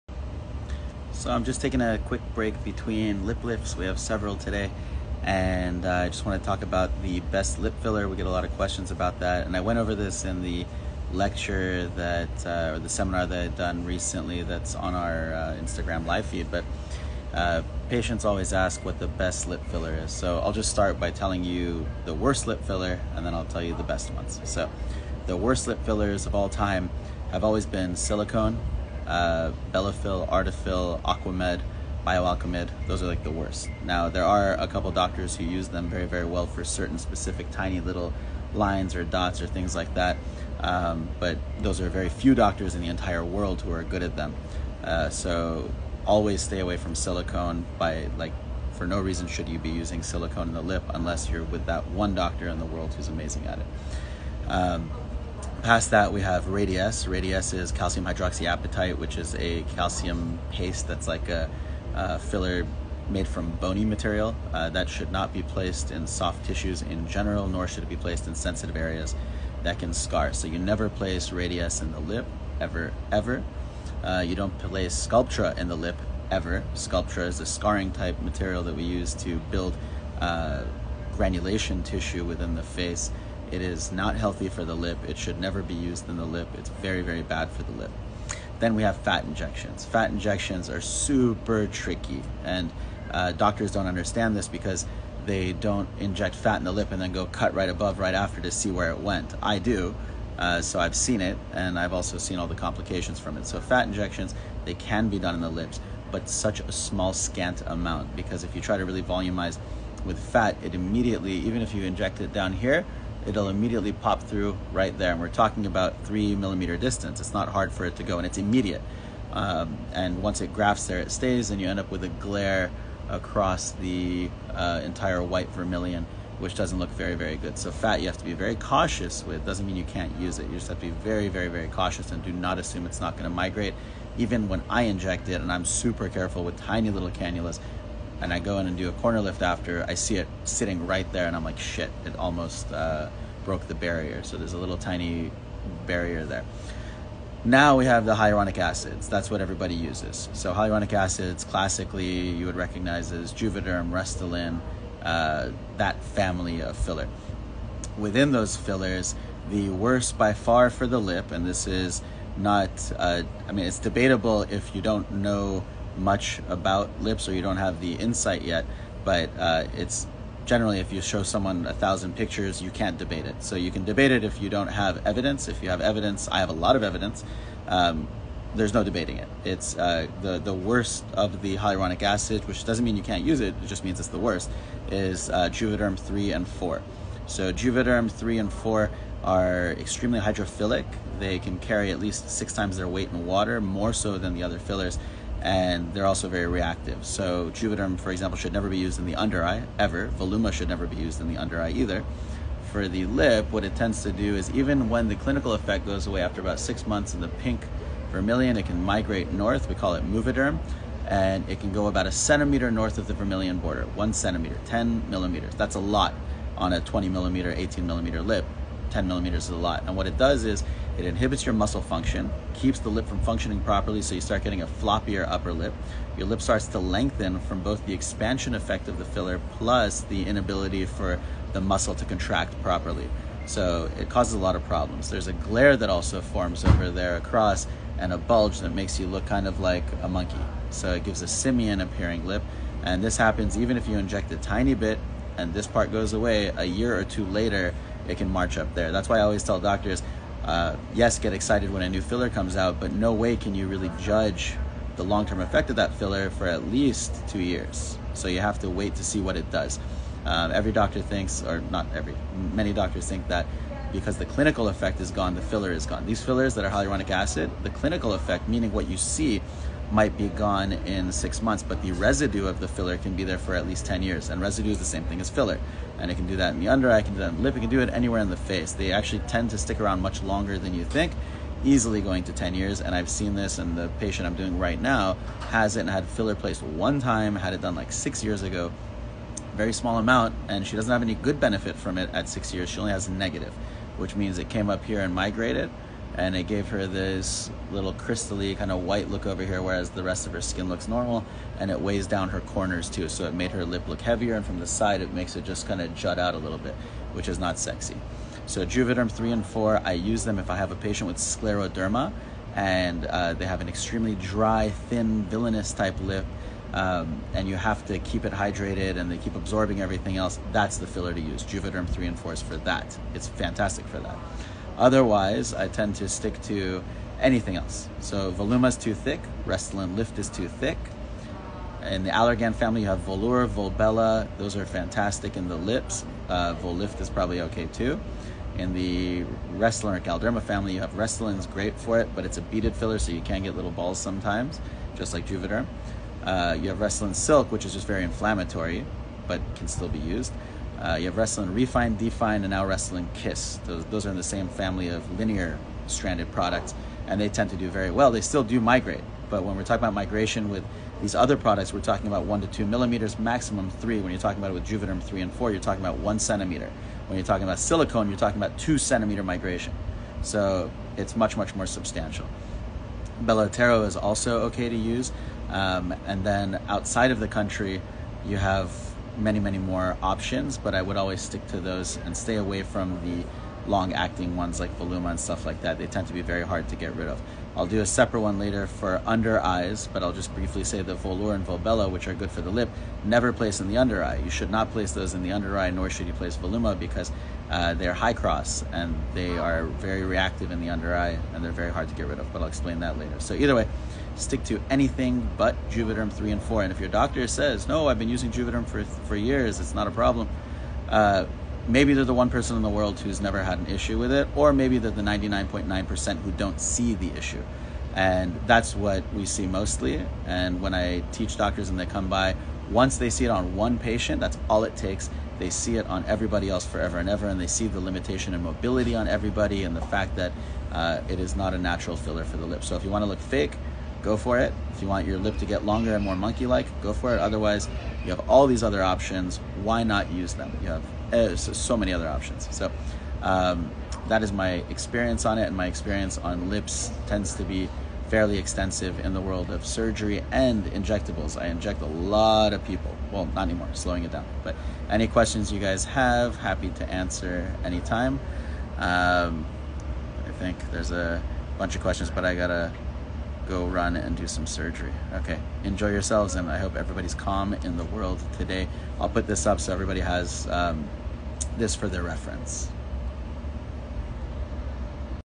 The Best Lip Filler | Lesson Of The Day
I get asked this all the time! I used to do 20-50 Injectables a day and I do lip surgeries pretty much every single day so I get to see the lip inside and out. This means that my opinions are not based on theory, Hopes and dreams. They are based on actual physical⁣ findings. Which is why so many doctors still using Juvederm in the lips don’t agree with me - because they haven’t seen things in the right light. Anyone who listens, sees the issues immediately. The injector is very important but so is the product. We are alive and our bodies react. Hope this helps. Sorry if I didn’t go into more detail but had to run to surgery⁣.

Beverly Hills Center for Plastic Surgery: Dr. Ben Talei